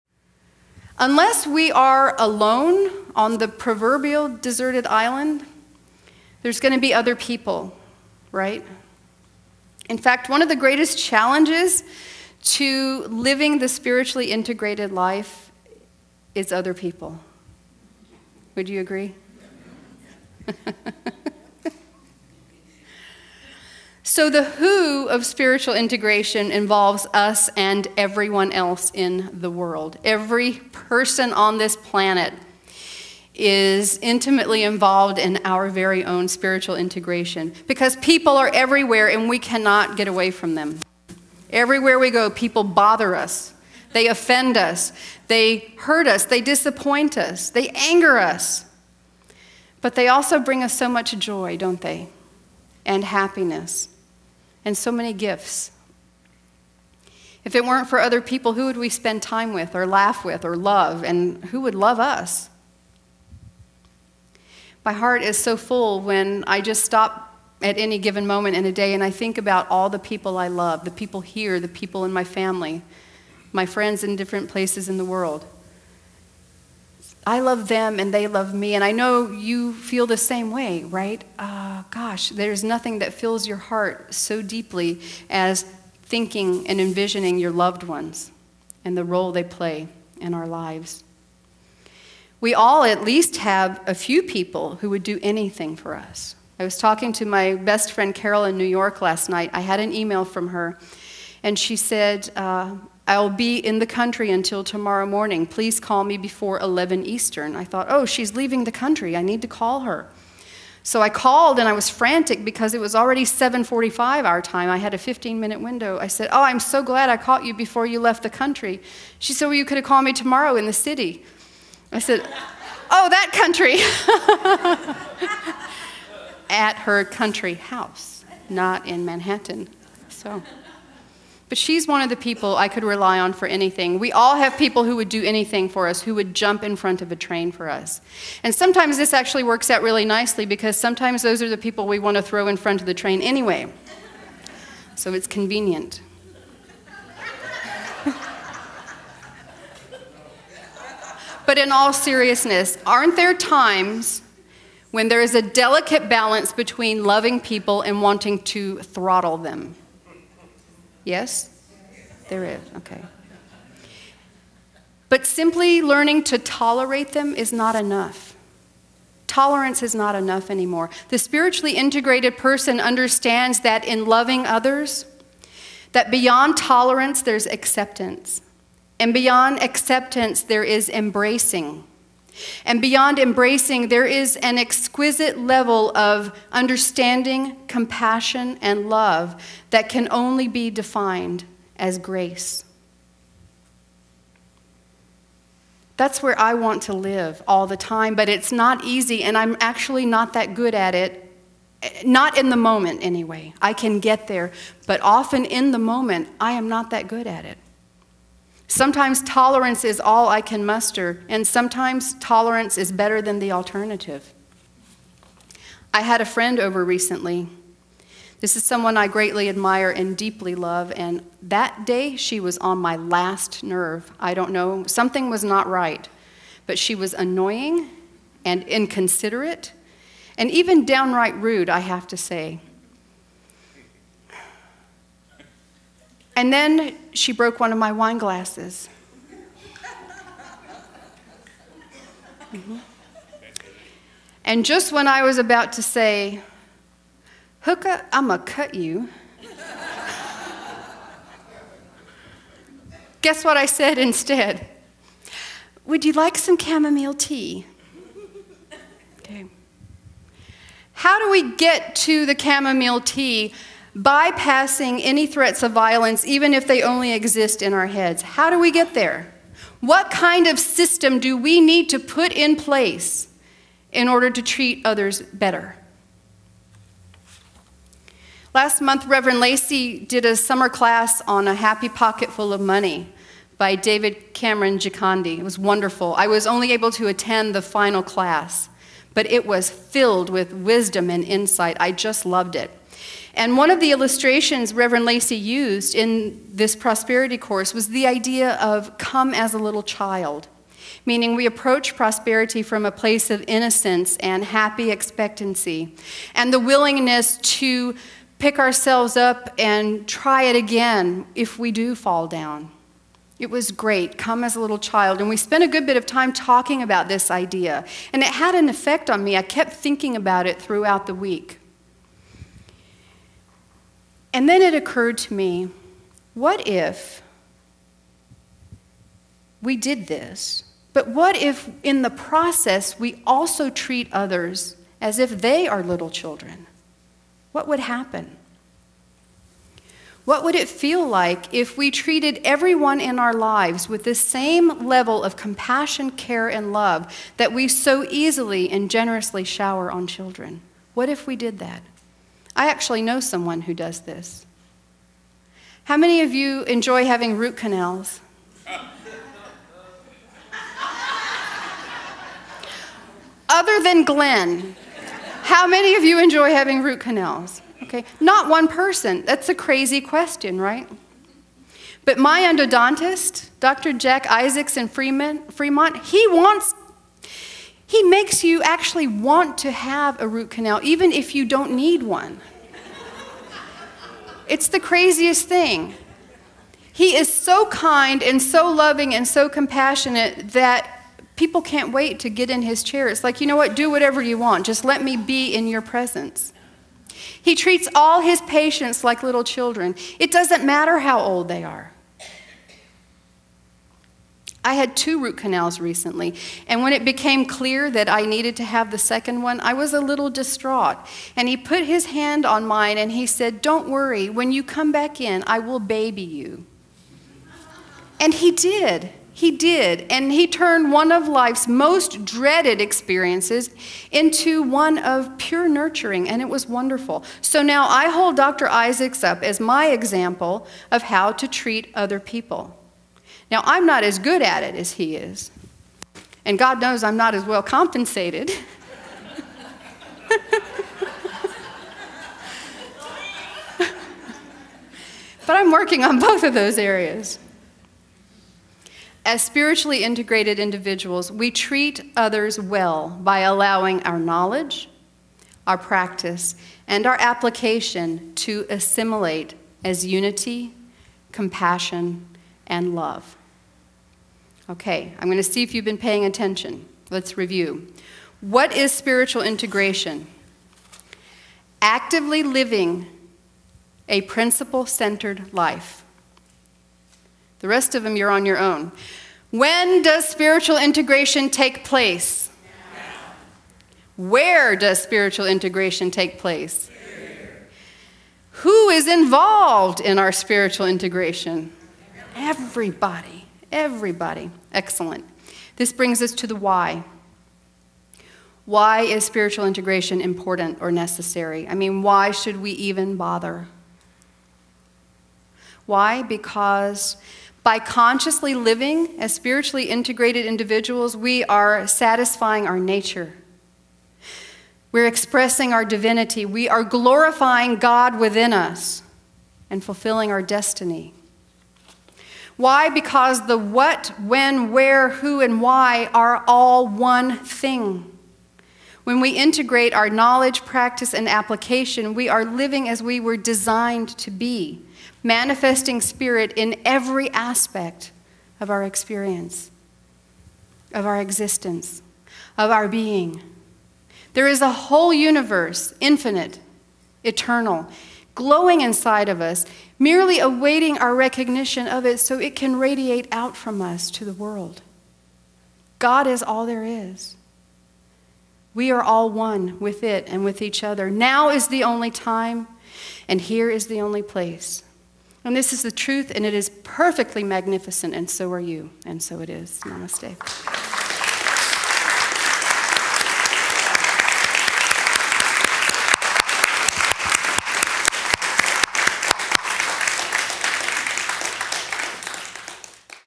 Spiritual Integration, Part 3.mpg
Rev. Dr. Cynthia Cavalcanti of the Center for Spiritual Living, Fremont, California speaking on Sunday, August 15, 2010 - "Spiritual Integration" Part 3 of 3